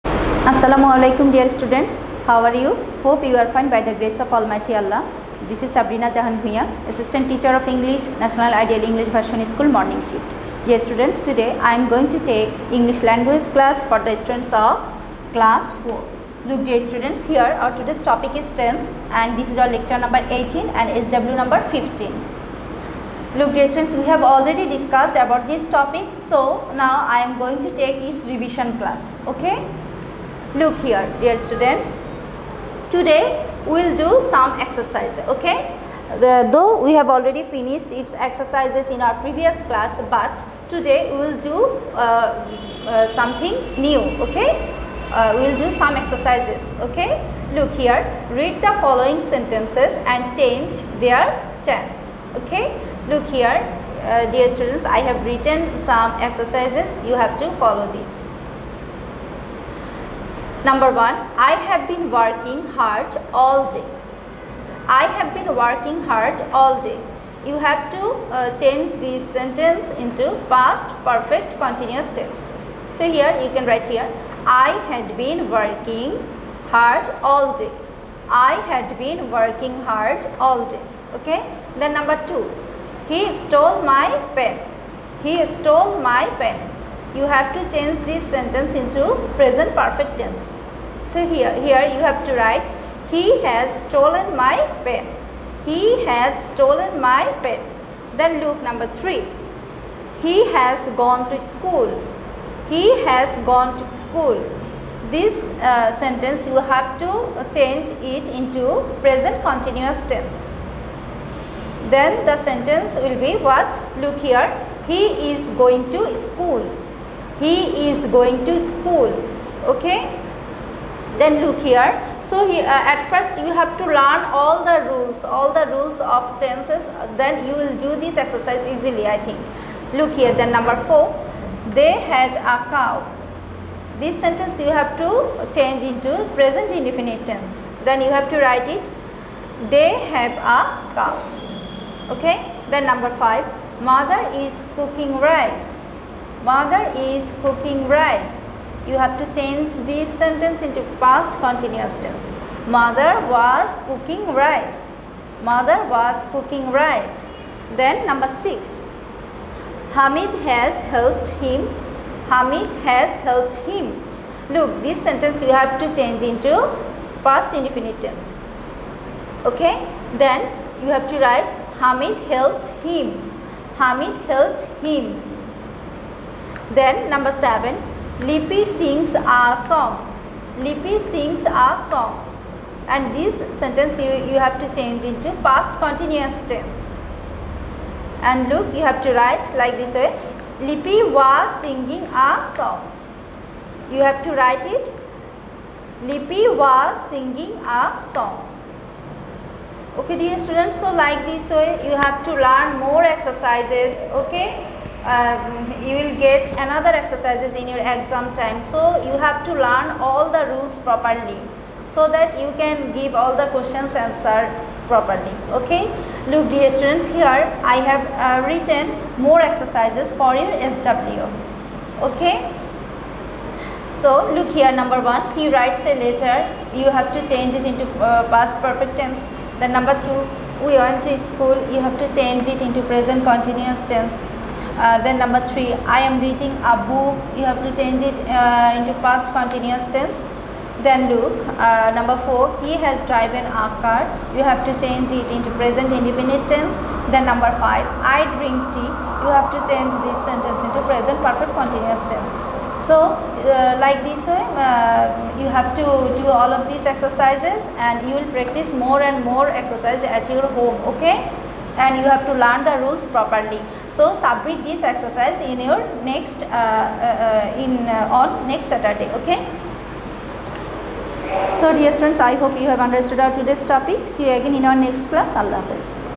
Class: Four, Subject: English Language (Lecture- 18), Topic: Tense (R.W)
Class: Four
Subject: English Language (Lecture- 18)
Topic: Tense (R.W)
Teacher: Sabrina Jahan Bhuiyan